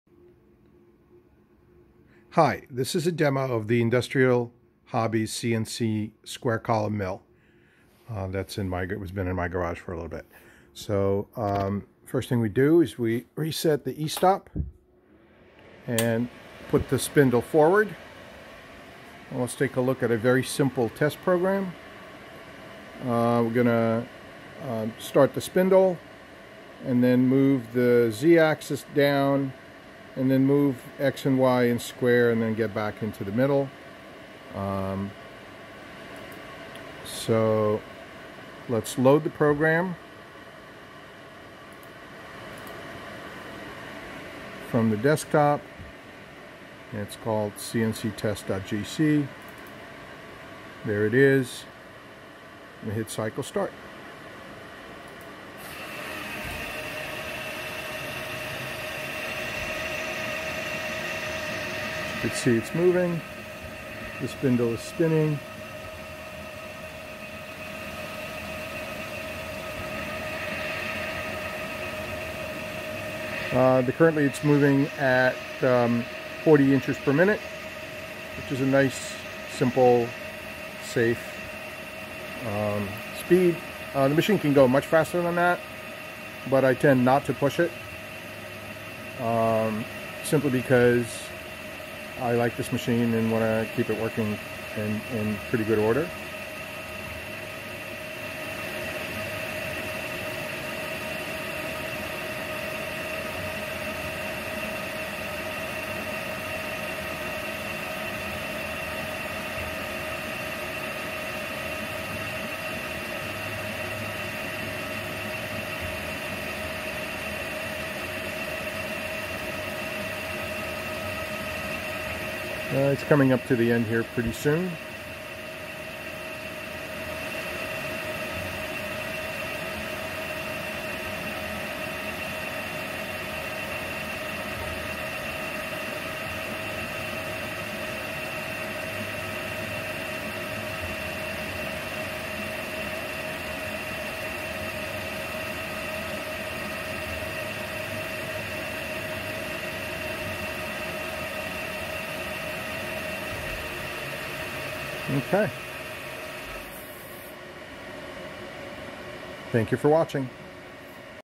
Industrial Hobbies CNC Mill
A demo of the mill in motion